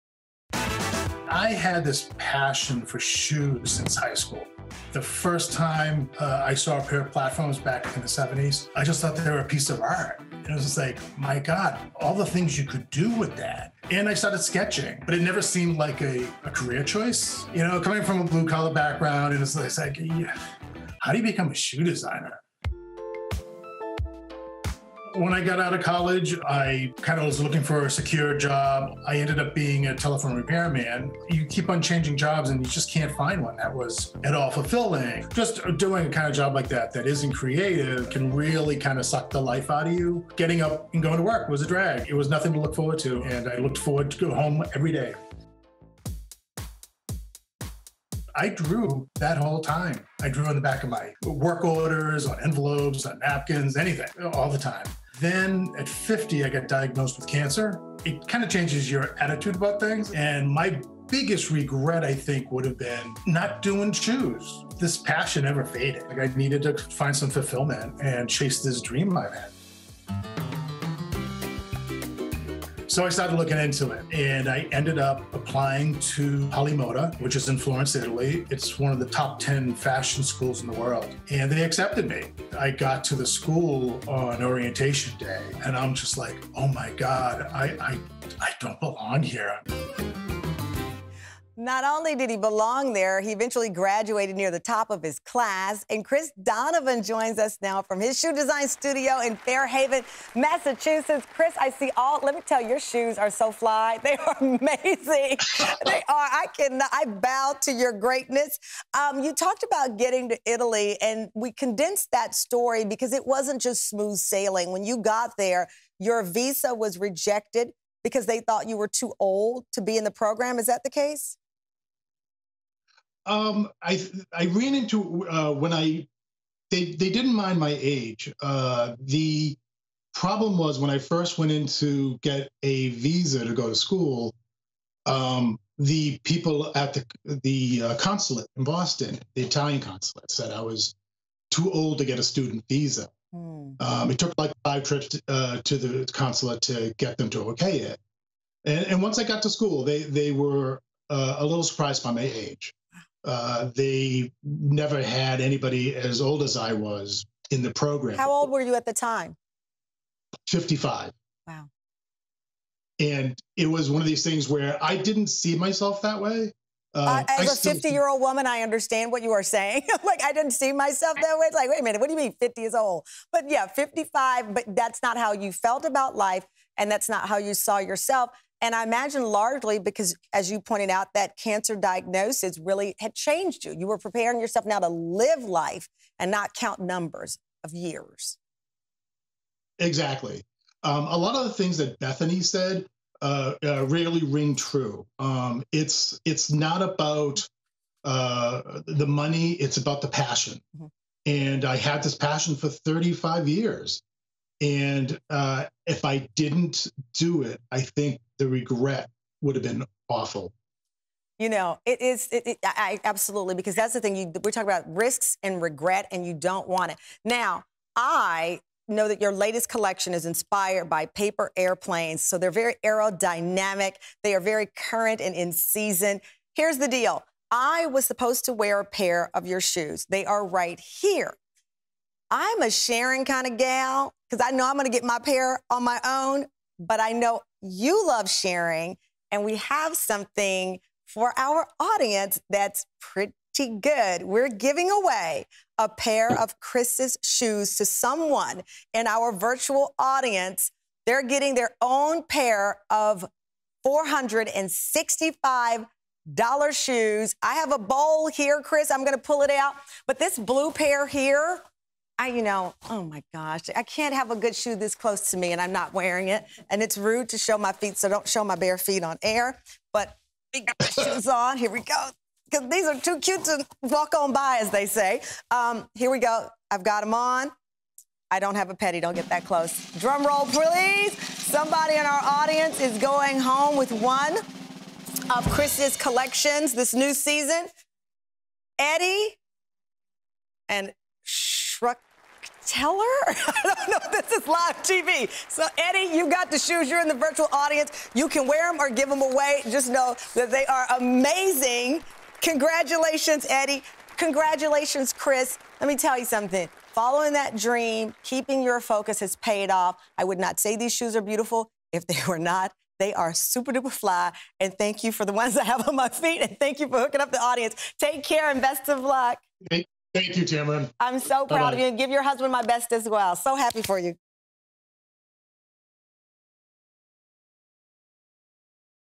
A Cancer Diagnosis Caused This Telephone Repair Man to Risk It All & Become A Shoe Designer
Chris Donovan might just be the next inspiration behind a blockbuster film. After nearly 30 years on the workforce as a telephone repair man, Chris uprooted his life. He was diagnosed with prostate cancer at 50 and at that moment, decided to pursue his passion for designing shoes. Since then, he was accepted to Polimoda, one of the best fashion design institutes in the world, and finally launched Chris Donovan Footwear.